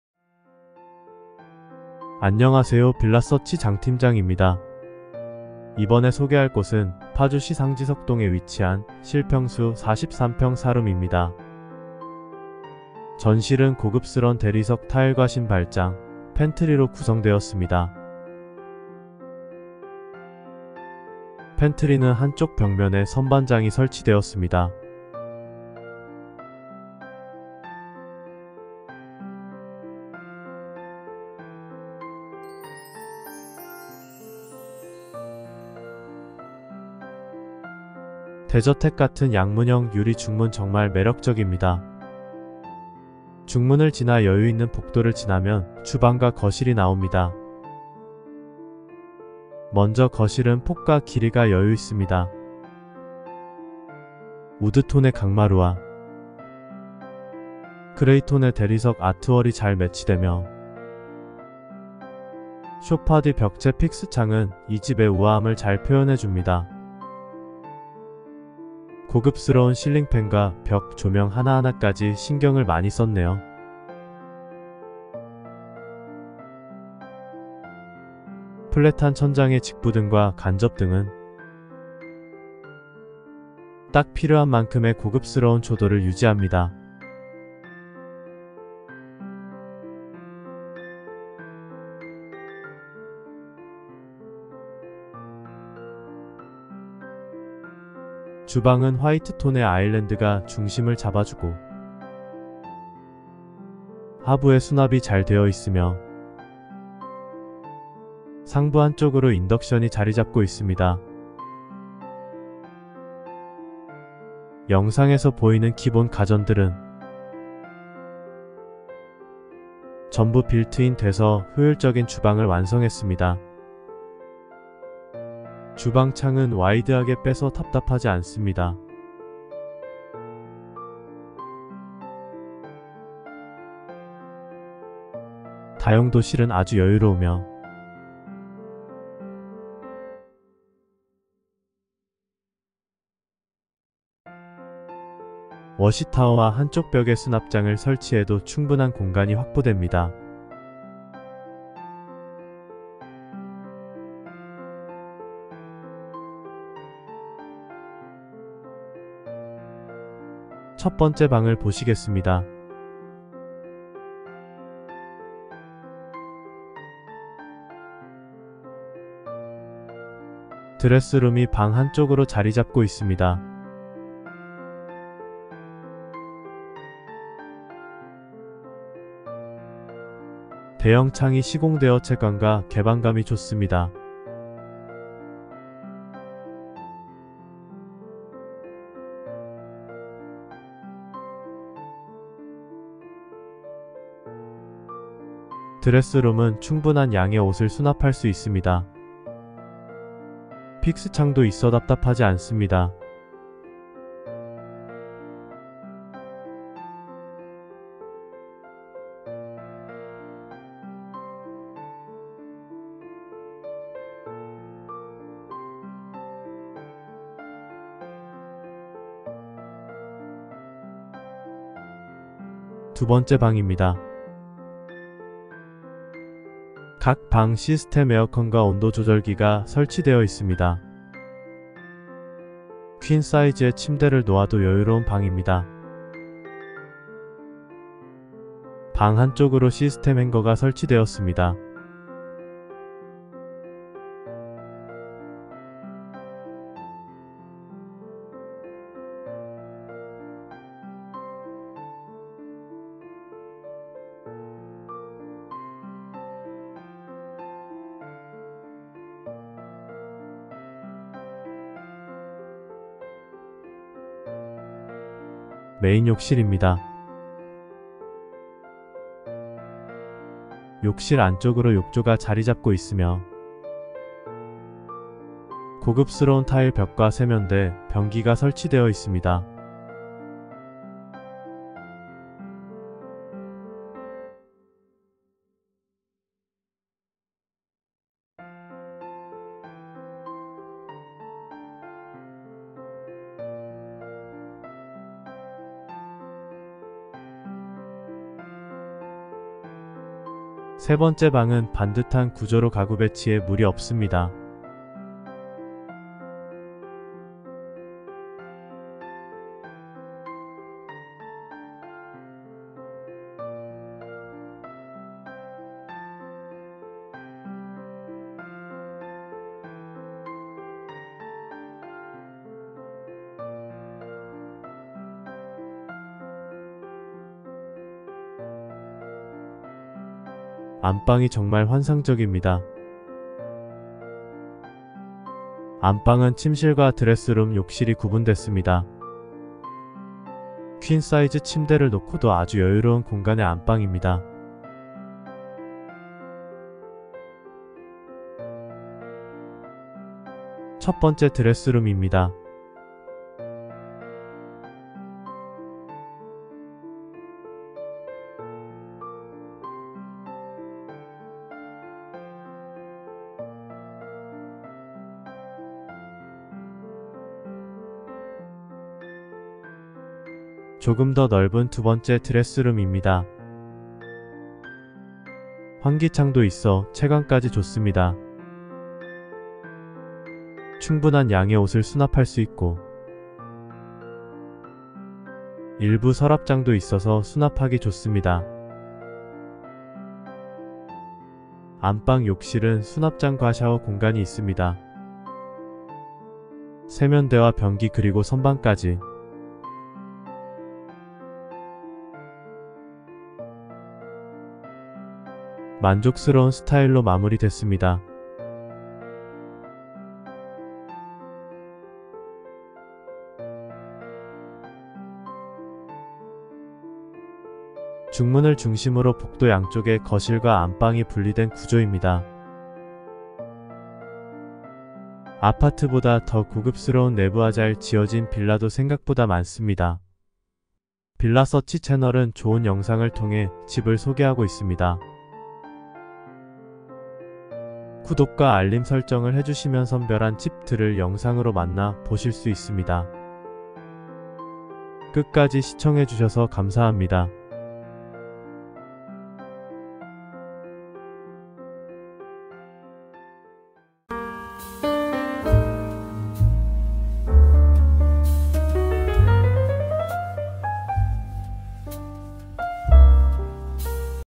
[ NO.1050 ] 파주 신축빌라 빌라에서는 찾아보기 힘든 전망 좋은 대형 사이즈 4룸 신축빌라.
파주 신축빌라 빌라에서는 찾아보기 힘든 전망 좋은 대형 사이즈 4룸 신축빌라.

매물위치 : 파주시 상지석동
분양세대 : 2개동 / 24세대
면적정보 : 전용면적 25.04PY / 실내면적 43.29PY
건축타입 : 방4 + 욕실2
기본옵션 : 에어컨 4대, 광파오븐, 인덕션, 식기세척기, 냉장고등
분양가격 : 문의주세요